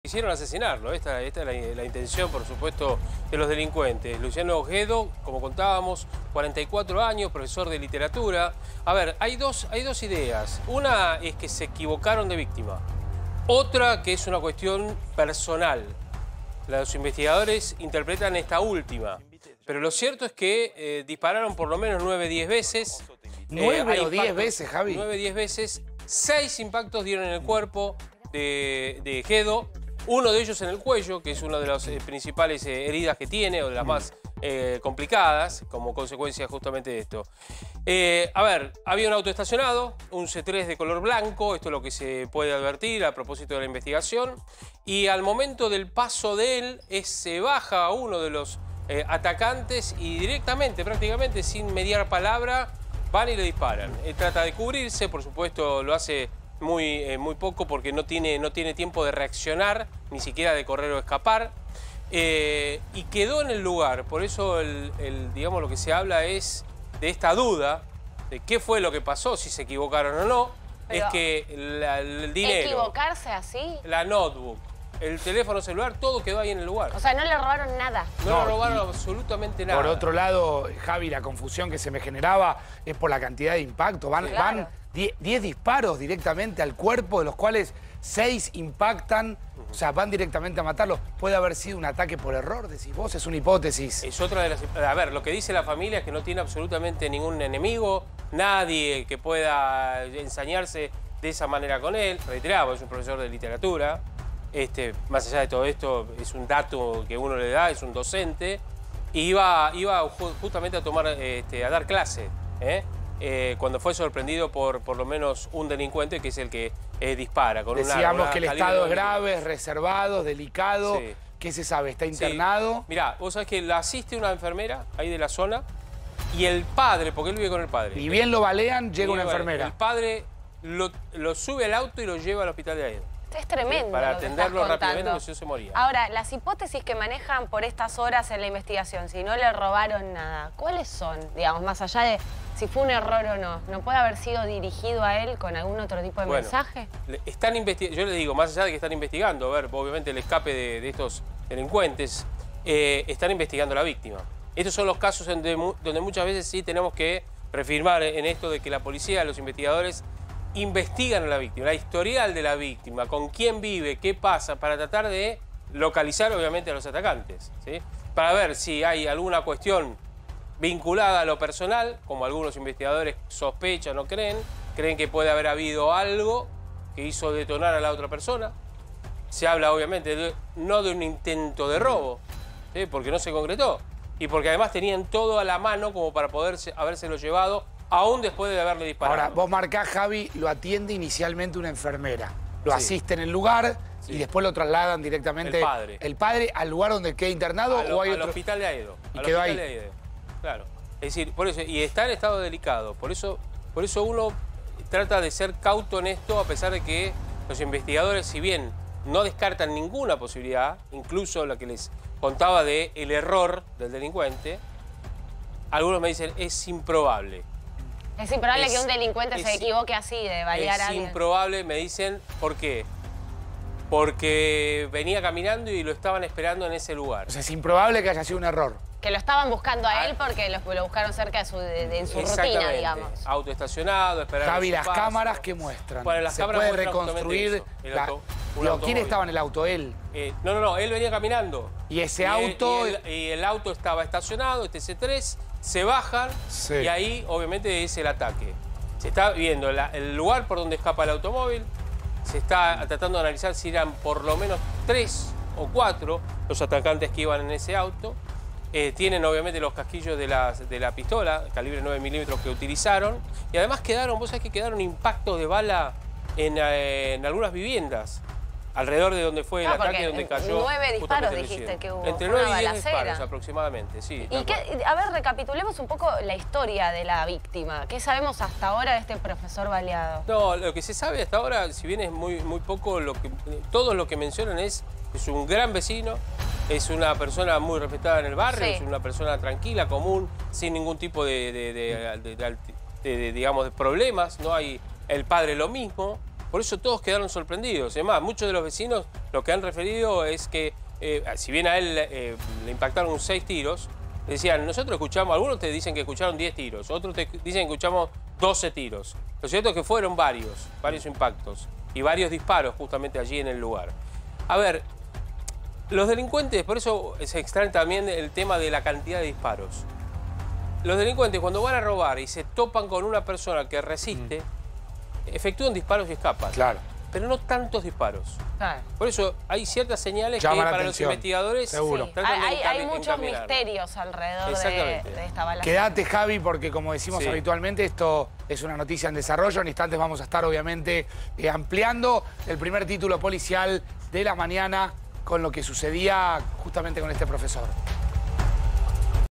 🚨 Profesor baleado: ¿error o pase de factura? | A24
👉 Javier Díaz contó la historia del docente de literatura que recibió seis disparos y está internado en grave estado.